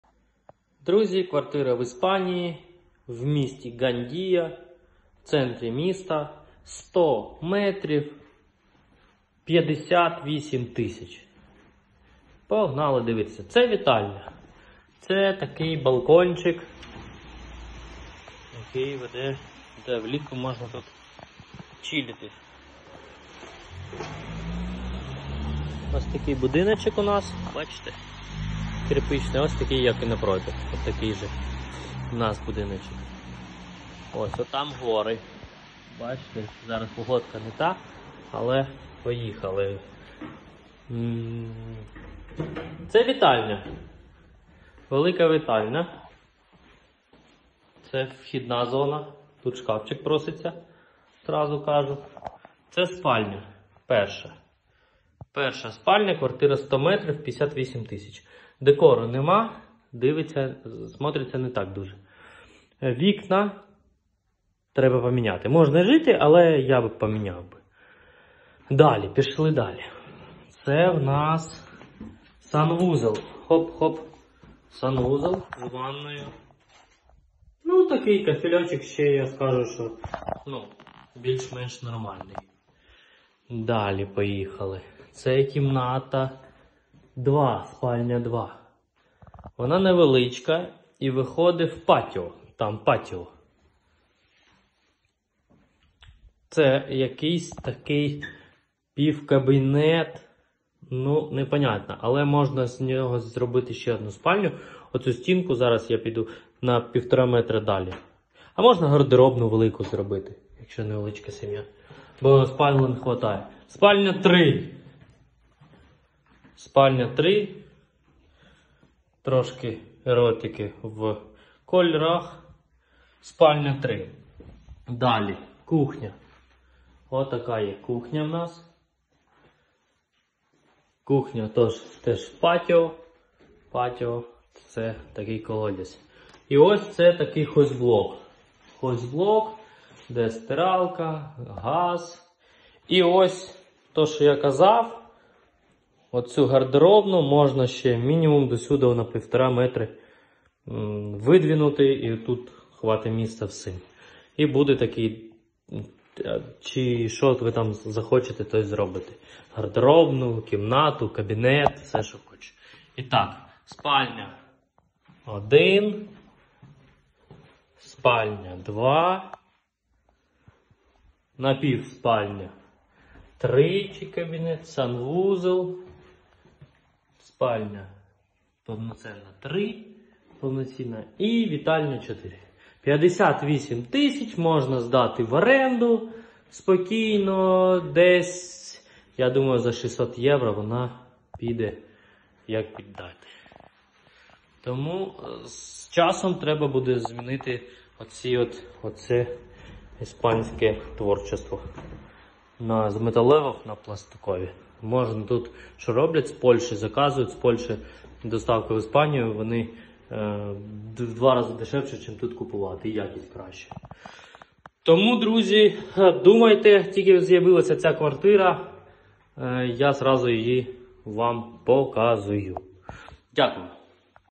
ДЕШЕВА НЕРУХОМІСТЬ В ІСПАНІЇ
Цікава НЕРУХОМІСТЬ В ІСПАНІЇ 🇪🇸

✅🫰- ВСЬОГО 58.000€
✅Знаходиться в місті, до моря біля 3 км
✅GANDIA(Valencia)-це місто недалеко від Валенсії,яке розташоване на узбережжі  моря, де знаходиться самий ❇️найкращий❇️ пляж в Іспанії!
✅ Площа - 100 м2
✅ Поверх - 4 без ліфта
✅  3 кімнати + вітальня
✅Переоформлення квартири - 8.000€ «Під ключ»
✅Вкласти в ремонт ~ 1000€ -5000€
✅ Оренда 600€ в місяць!

І у Вас завжди є «запасний аеродром»